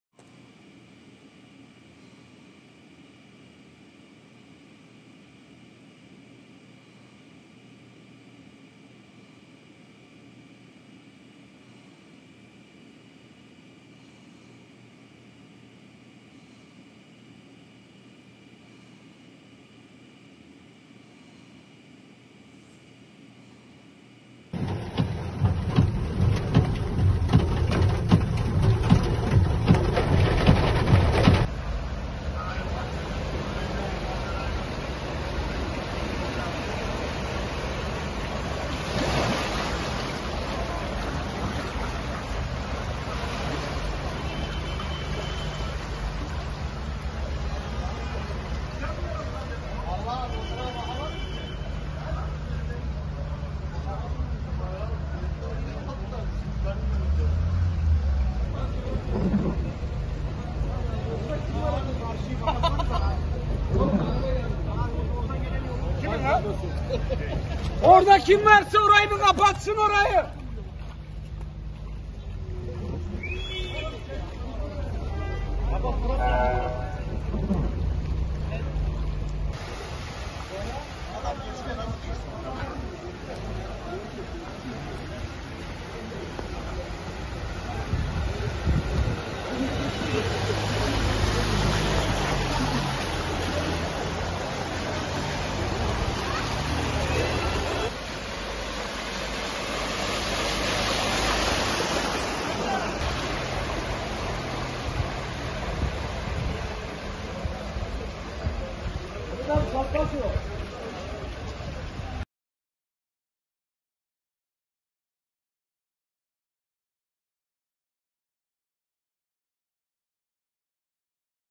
Trafik Polisi Sel Sularında Vatandaşı Sırtına Alarak Kurtardı!
Yozgat Yerköy'de akşam saatlerinde etkili olan sağanak sel felaketine neden oldu. Bir trafik polisi, sel sularıyla kaplı yolda mahsur kalan bir vatandaşı sırtına alarak güvenli alana taşırken, bir diğer polis memuru ise selde mahsur kalan aracı iterek trafiğin açılmasına yardımcı oldu.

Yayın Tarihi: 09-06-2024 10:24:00